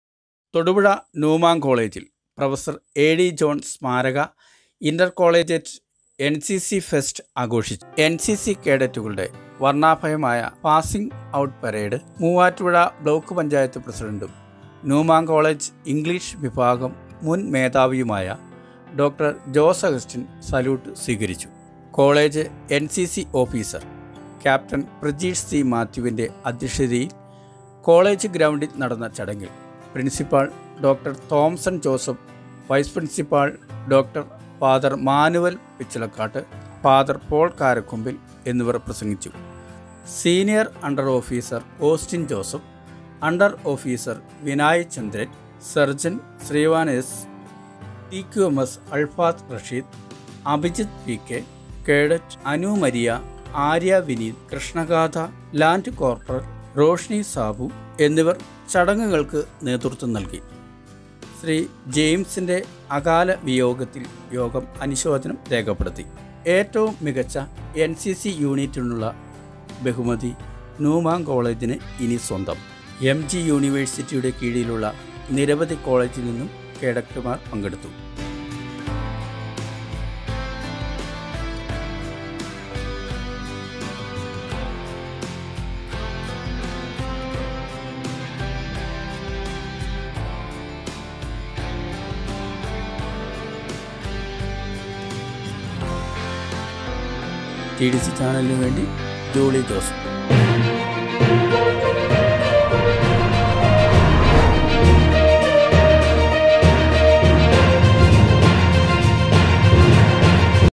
തൊടുപുഴ ന്യൂമൻ കോളേജിൽ പ്രൊഫ. എ.ടി. ജോൺ സ്മാരക ഇന്റർ കോളേജിയേറ്റ് എൻ.സി സി ഫെസ്റ്റ് ആഘോഷിച്ചു
എൻ. സി.സി.കേഡറ്റുകളുടെ വർണ്ണാഭമായ പാസ്സിംഗ് ഔട്ട് പരേഡിൽ മൂവാറ്റുപുഴ ബ്ലോക്ക് പഞ്ചായത്ത് പ്രസിഡന്റും ന്യൂമൻ കോളേജിലെ ഇംഗ്ലീഷ് വിഭാഗം മുൻ മേധാവിയുമായ ഡോ.ജോസ് അഗസ്റ്റിൻ സല്യൂട്ട് സ്വീകരിച്ചു. കോളേജ് NCC ഓഫീസർ ക്യാപ്റ്റൻ പ്രജീഷ് C മാത്യുവിന്റെ അദ്ധ്യക്ഷതയിൽ കോളേജ് അങ്കണത്തിൽ നടന്ന ചടങ്ങിൽ പ്രിൻസിപ്പാൾ ഡോ. തോംസൺ ജോസഫ്, വൈസ് പ്രിൻസിപ്പാൾ ഡോ.ഫാദർ മാനുവൽ പിച്ചളക്കാട്ട്, ട്രഷറർ ഫാദർ പോൾ കാരക്കൊമ്പിൽ എന്നിവർ പ്രസംഗിച്ചു, ന്യൂമൻ കോളേജ് സംസ്ഥാനത്തെ ഏറ്റവും നല്ല എൻ. സി.സി യൂണിറ്റിനുള്ള അവാർഡു നേടിയിരുന്നത് ഇത്തരുണത്തിൽ ഓർമ്മിക്കുമല്ലോ…
   സീനിയർ അണ്ടർ ഓഫീസർ ഓസ്റ്റിൻ ജോസ്,
 അണ്ടർ ഓഫീസർ വിനയ് ചന്ദ്രൻ, സെർജന്റ് ശ്രാവൺ. എസ്
CQMS അൾത്താ… TDCKERALA jolly joseph